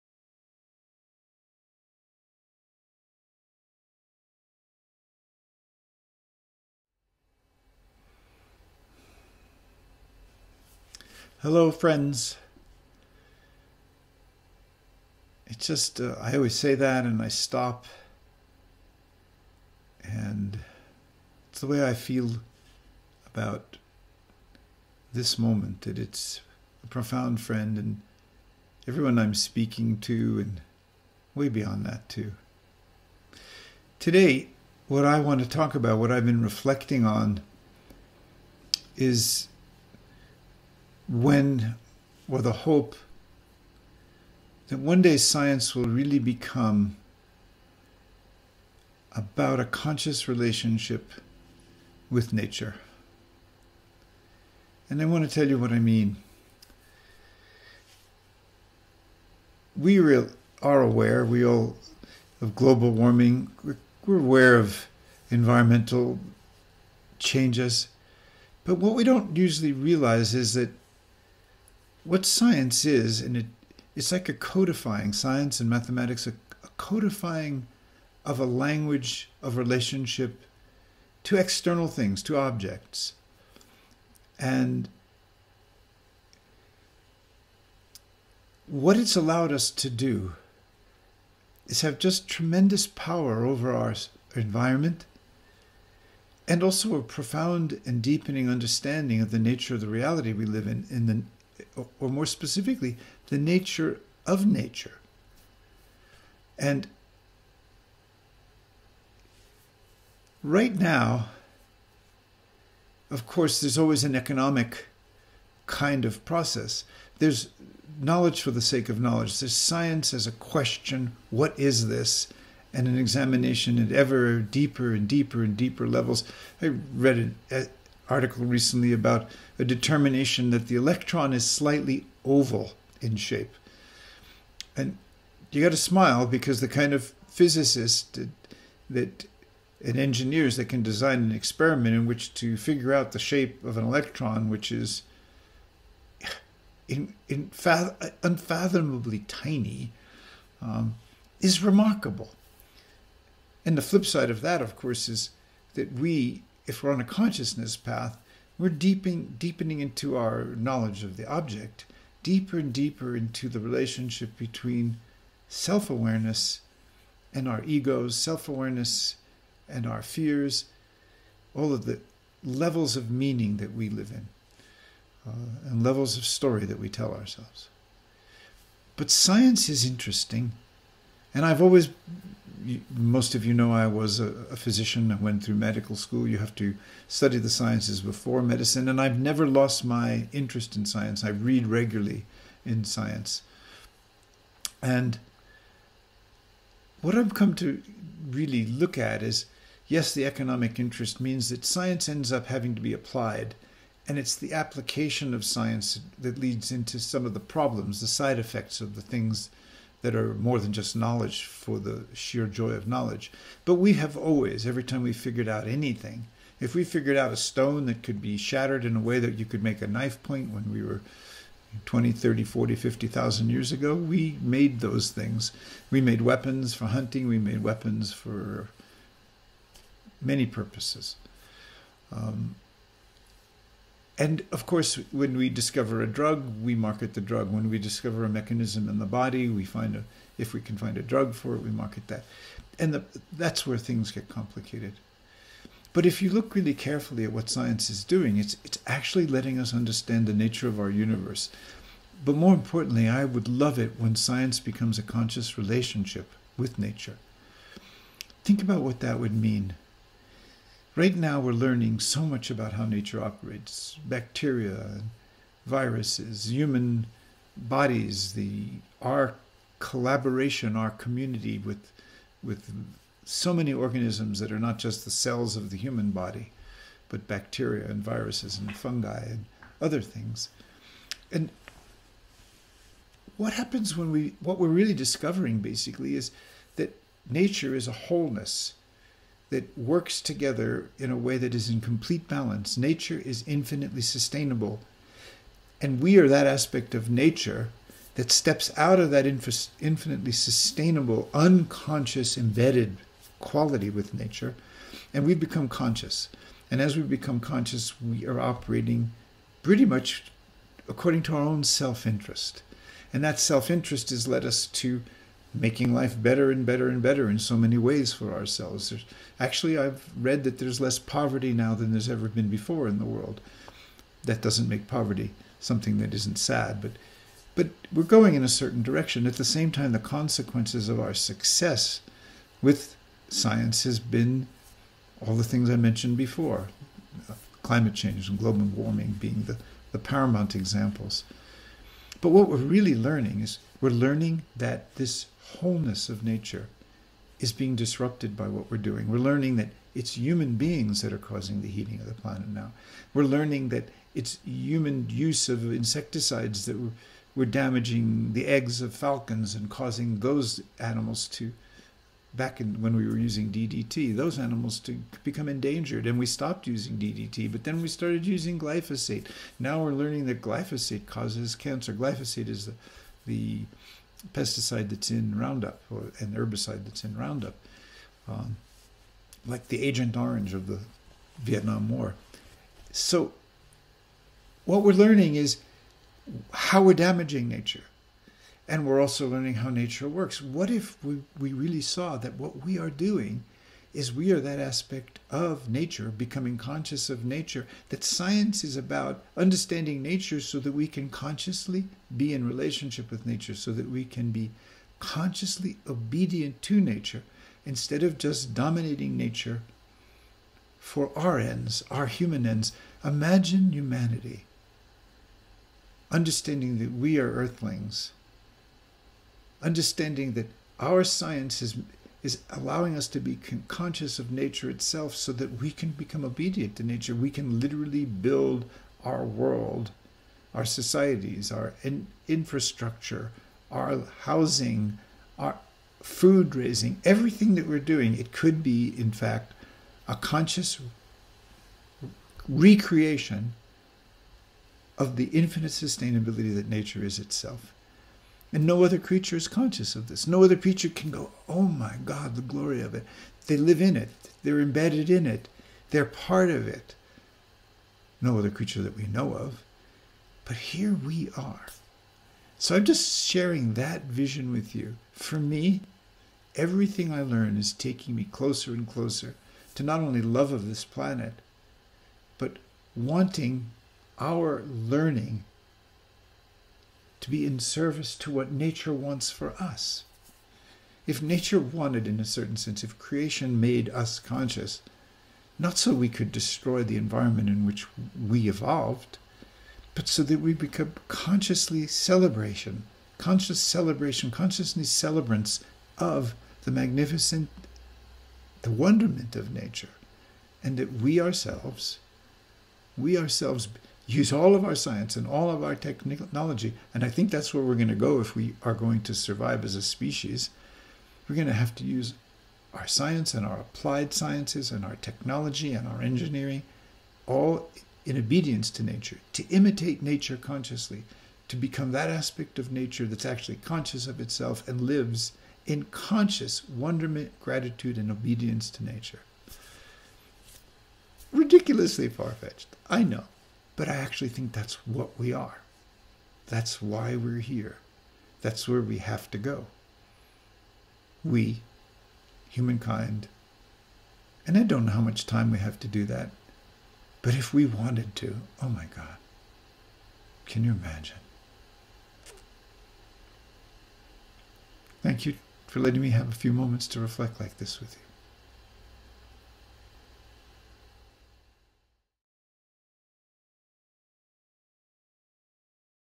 Science and Nature
What would it be like if science was practiced as a conscious relationship with Nature?